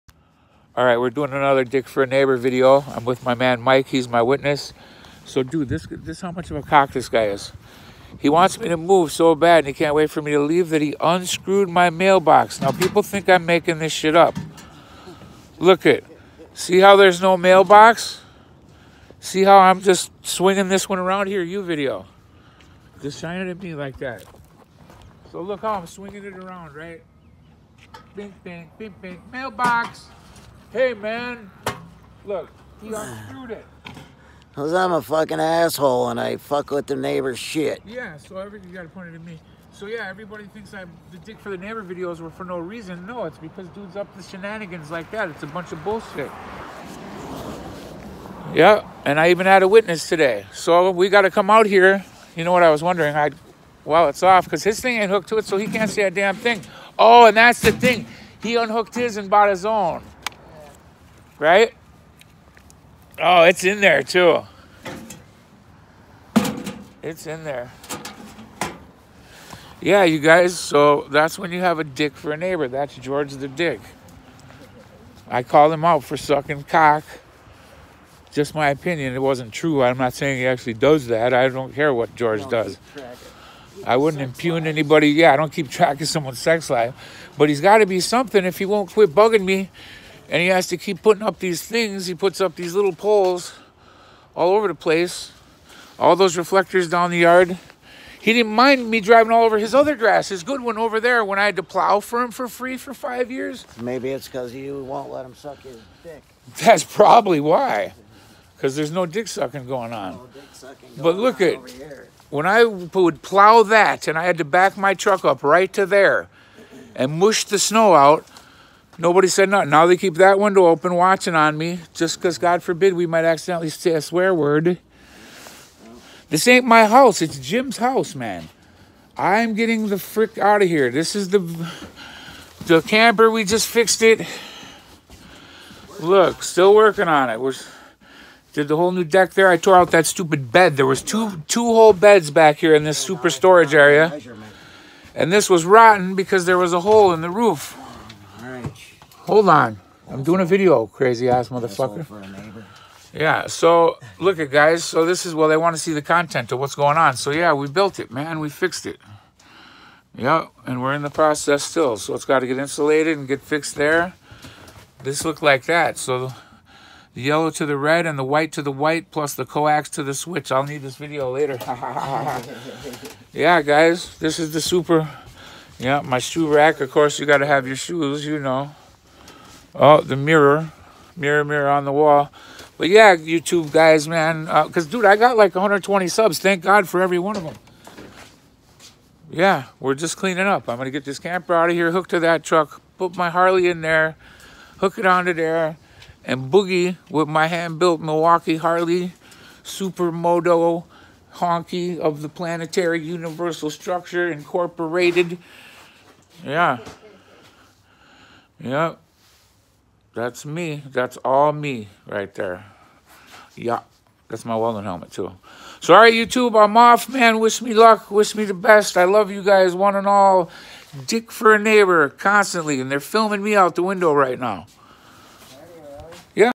D@CK 4 NEIGHBOOR VLOG! He KILLED MY MAILBOX!
So of course I guess he had to buy his own stand and put it on there and separated from mine so they weren’t touching. Or close to being touched. So yeah he yeah he did that and that he unscrewed mine and I bolted in the process I guys as a final F off to see you later because he’s a little weak ass and I was I’m moving bought it all voluntarily of my old volitional purpose like with a purpose. So I don’t understand the animosity like I said he didn’t care when I did plowing for decades for free or you guys remember lecture and subscribe if you like what I do here I have a patriarch and I also have a go fund me account if you wanna. Therefore we do it if you don’t please do not have a good one you guys i’ve got bless you all!!!